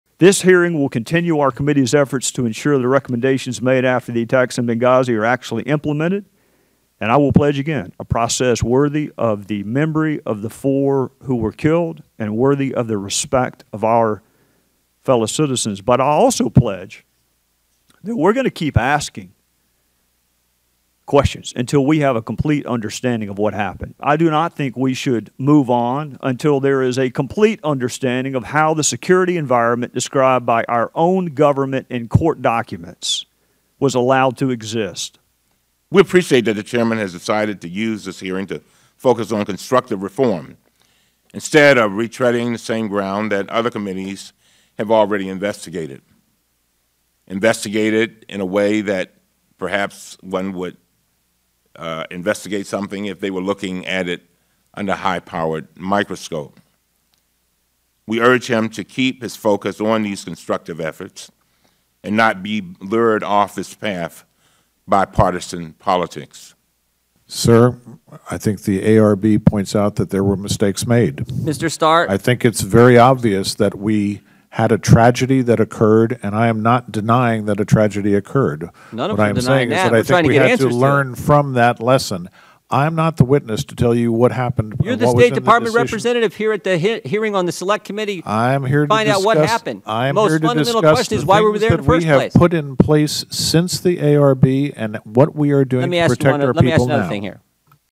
Gowdy: Will Keep Asking Questions on Benghazi
A special House committee is reviewing efforts to secure U.S. diplomatic facilities and personnel as it continues investigating the deadly 2012 attacks in Benghazi, Libya. (Dec. 10)